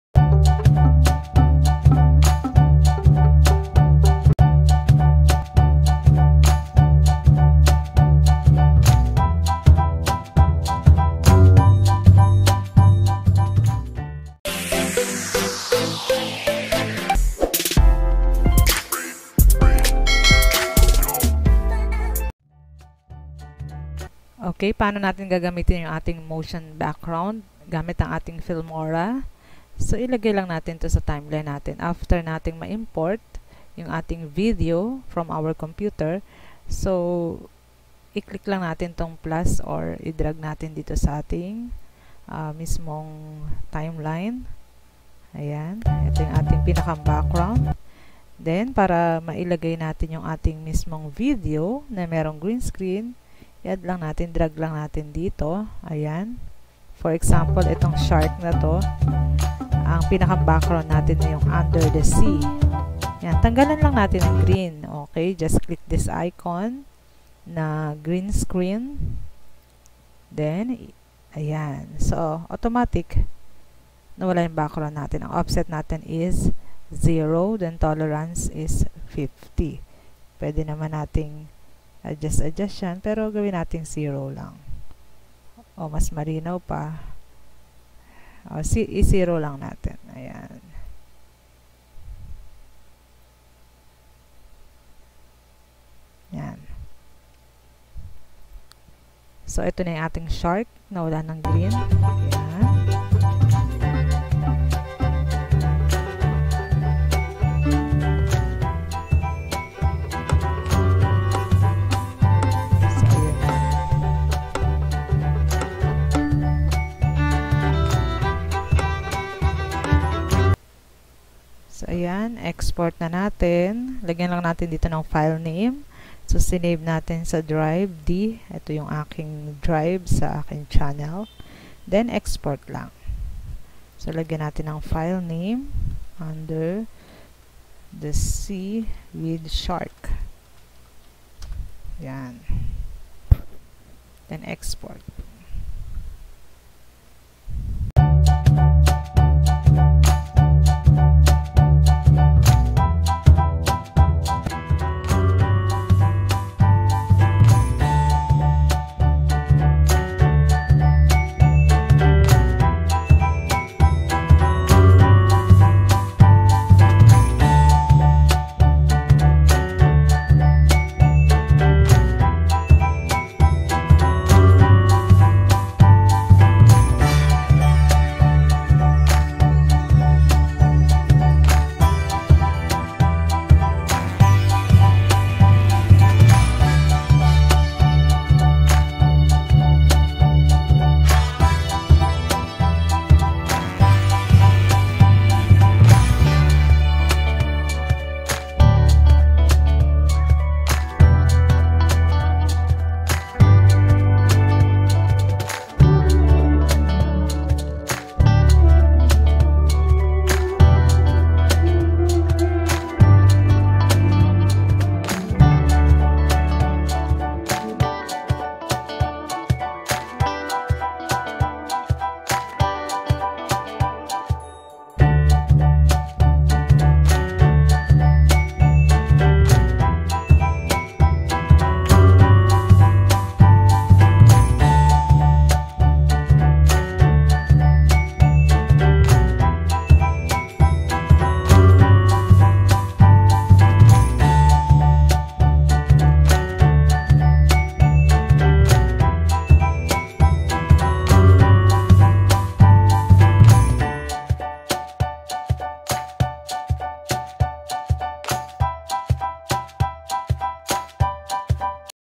FREE Motion Graphics (Under The Sea) with Tutorial
You can use this Under the Sea Motion Graphics for your green screen videos as your background.